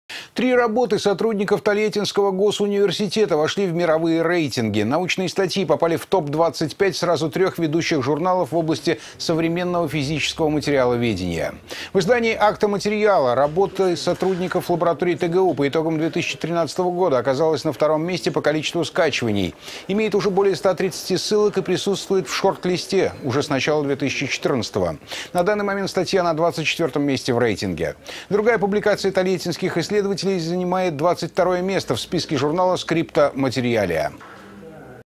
Три работы сотрудников ТГУ вошли в мировые рейтинги
Три работы сотрудников Тольяттинского госуниверситета вошли в мировые рейтинги. Научные статьи попали в TOP-25 сразу трёх ведущих журналов в области современного физического материаловедения. В издании Acta Materialia работа сотрудников лаборатории ТГУ по итогам 2013 года оказалась на втором месте по количеству скачиваний, имеет уже более 130 ссылок и присутствует в шорт-листе уже с начала 2014 года. На данный момент статья на 24 месте в рейтинге. Другая публикация тольяттинских исследователей занимает 22 место в списке журнала Scripta Materialia.